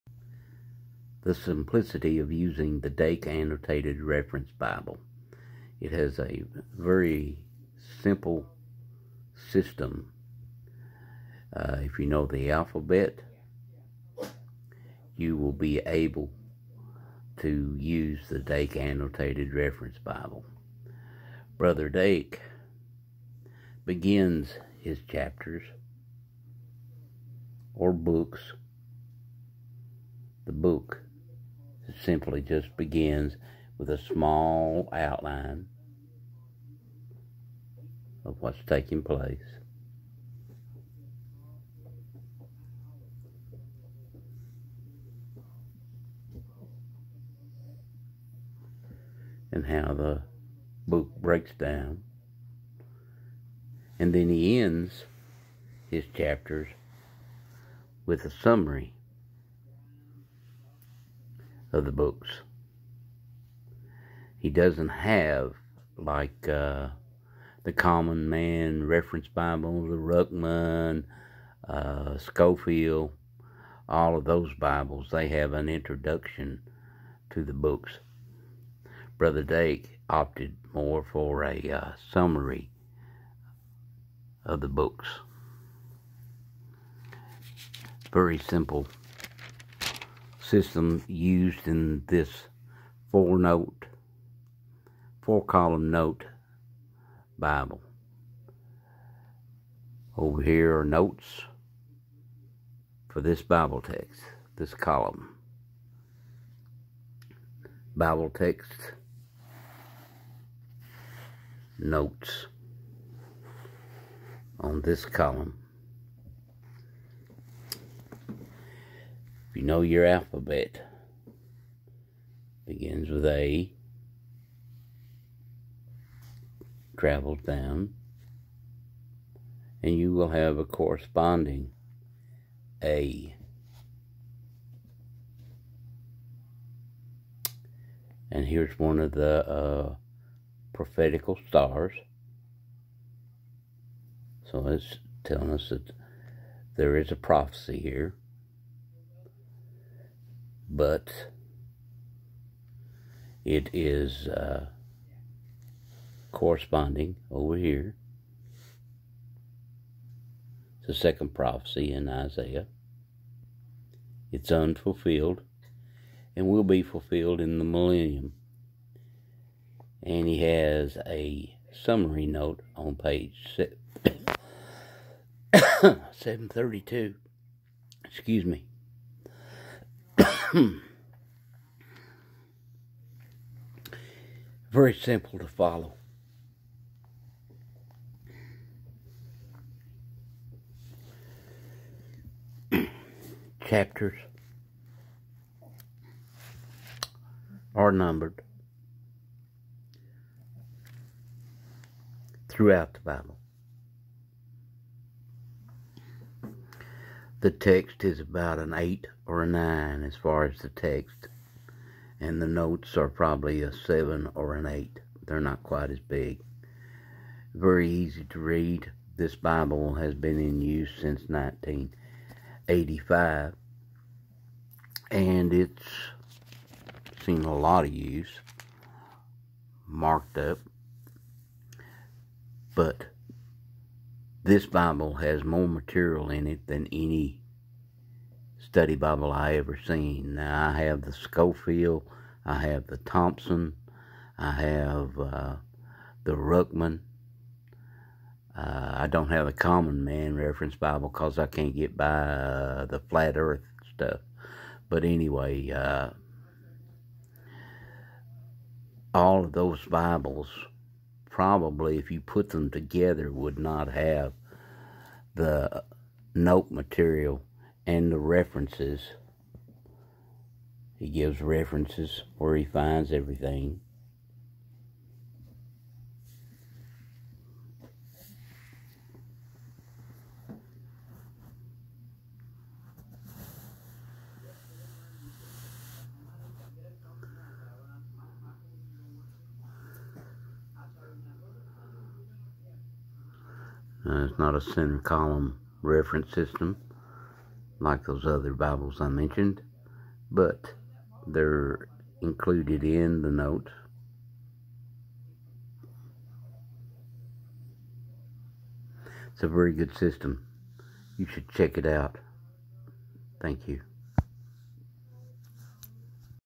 Using the Dake Annotated Reference Bible. (Simple)
The most extensive of KJV Study Bibles has the simplest Reference system devised.
Easy to use, and packed with information.